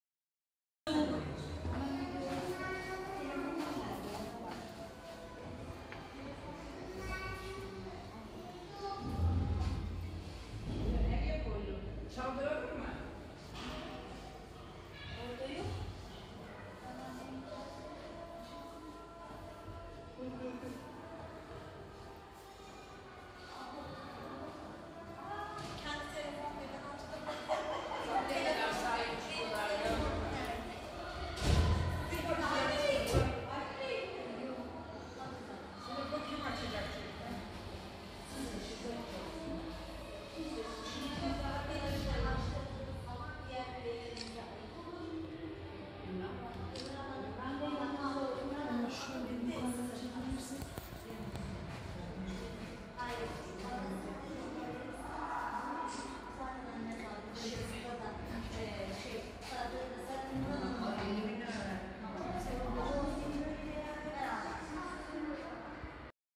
TKB Pendik Şubesi’nden Kütüphane Açılışı
TKB Pendik Şubesi, maddi desteği ile tefrişatını yapmış olduğu Esenyalı Turgut Reis İlkokulu’ndaki kütüphaneyi törenle açtı.